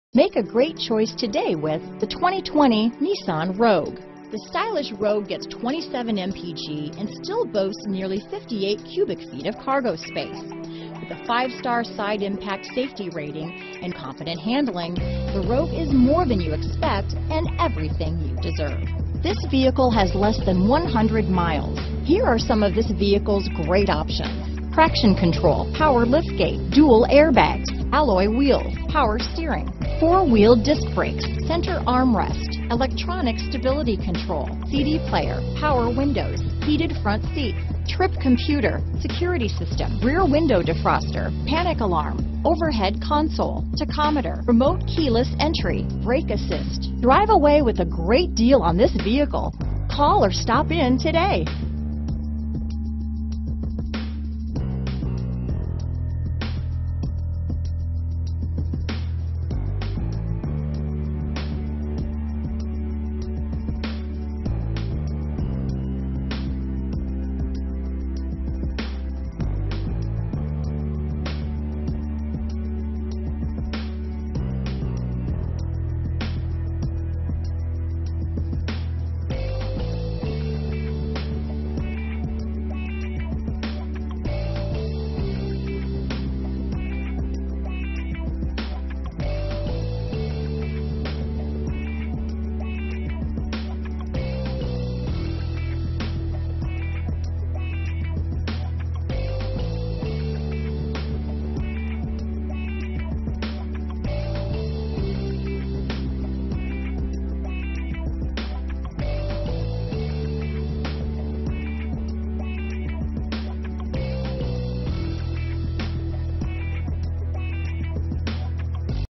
2020 Nissan Rogue Weatherford, Fort Worth, Granbury, Saginaw, Dallas, TX LW010351
Brilliant Silver Metallic New 2020 Nissan Rogue available in Fort Worth, Texas at Fort Worth Nissan. Servicing the Weatherford, Granbury, Saginaw, Dallas, TX area.

3451 West Loop 820 S

Brilliant Silver Metallic 2020 Nissan Rogue SV FWD CVT with Xtronic 2.5L I4 DOHC 16Vbrbr26/33 City/Highway MPG